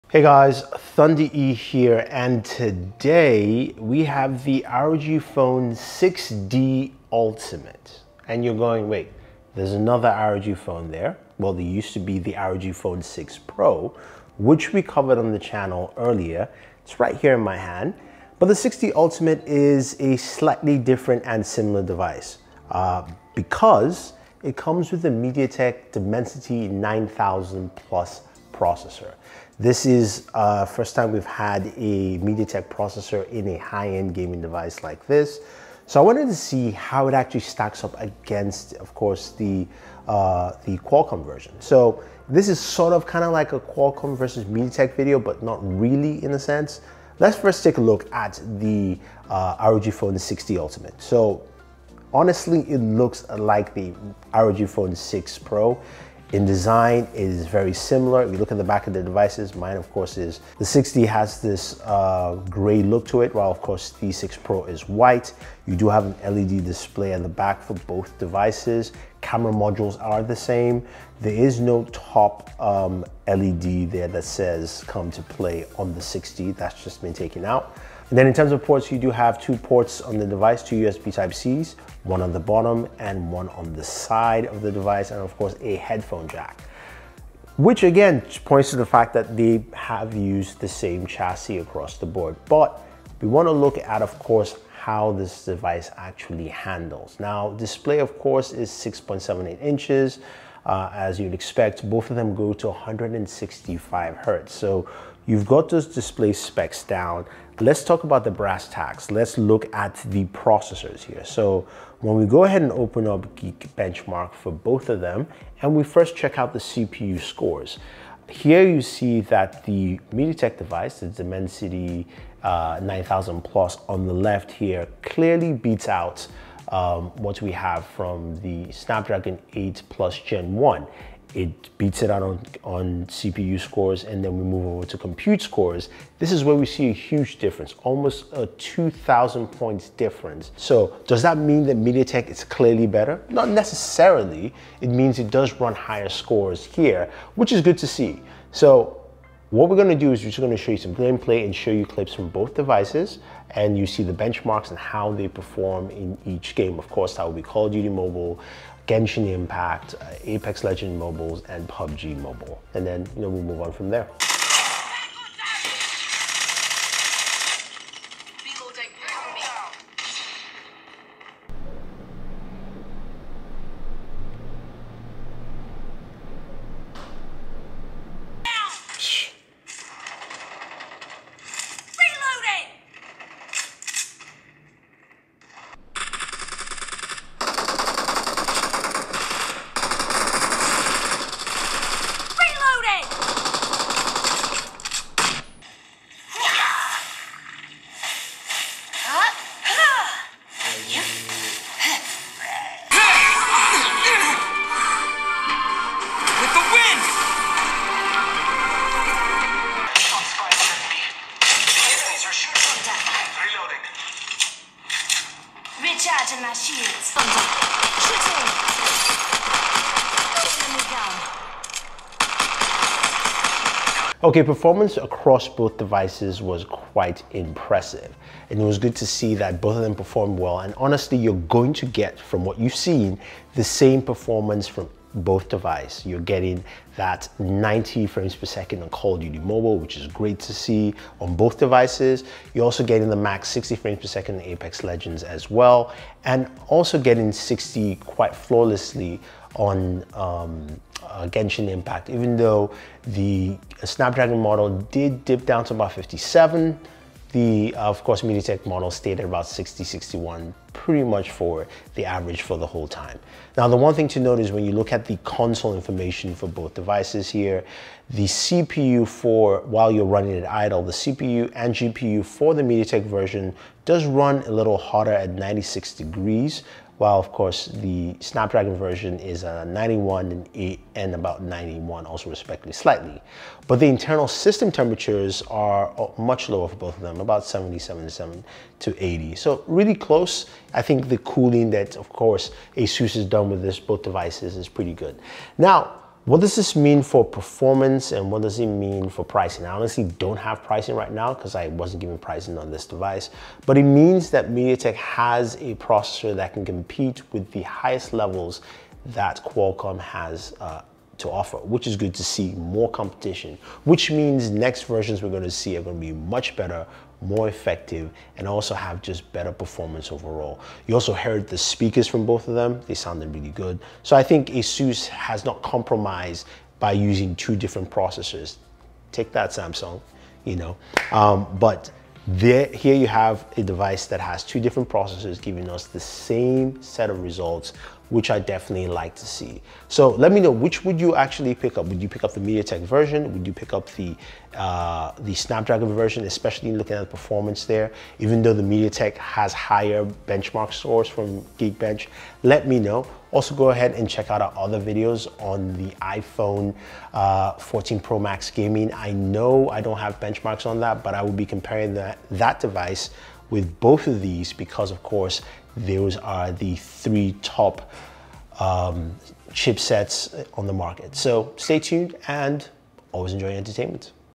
ROG Phone 6D Ultimate: A Mediatek Beast!
The new Asus ROG Phone 6D Ultimate is the lastest ROG gaming phone from Asus and this one is powered by the new Mediatek Dimensity 9000+ processor. so how well does it stack up against the Snapdragon Version.